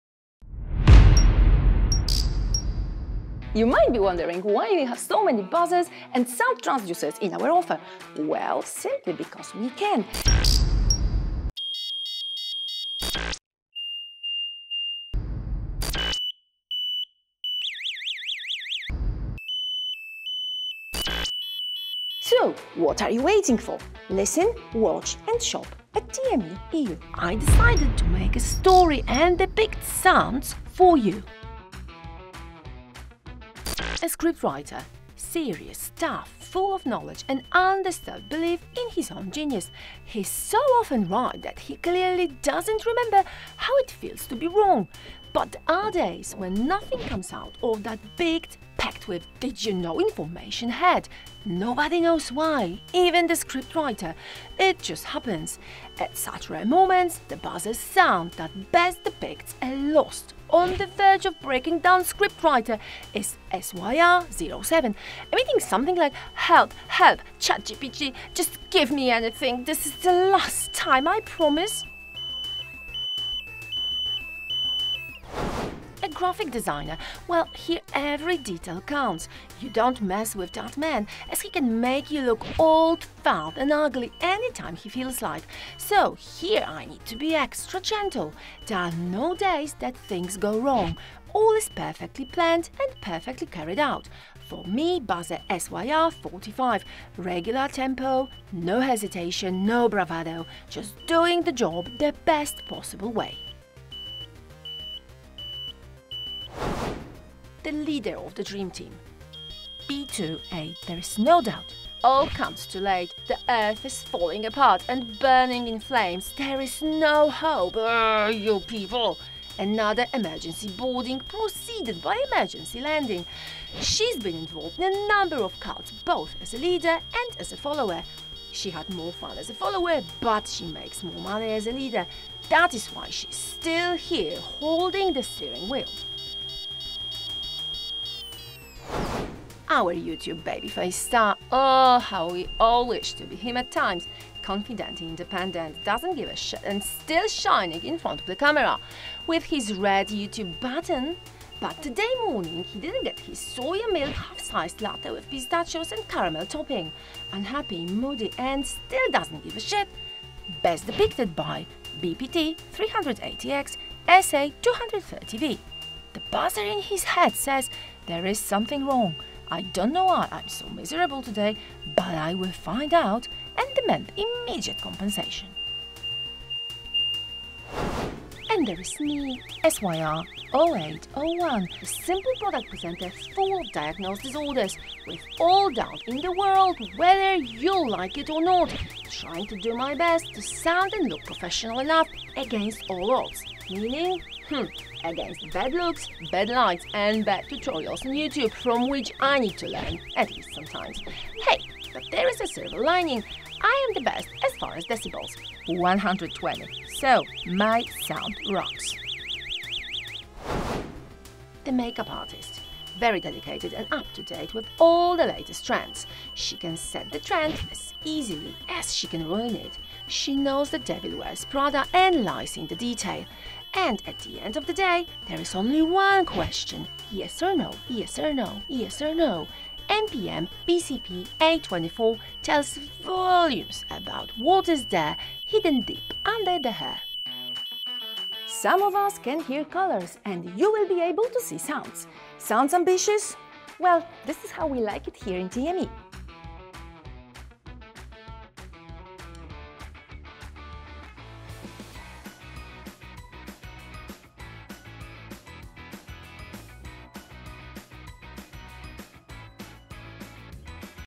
Every Sound Has Its Character! – Buzzers and Transducers Sounds Presentation
The characteristic sound of a buzzer or a sound transducer can really stimulate your imagination. And sometimes it might become real. In this video you will meet our characters that represent various buzzers from our offer.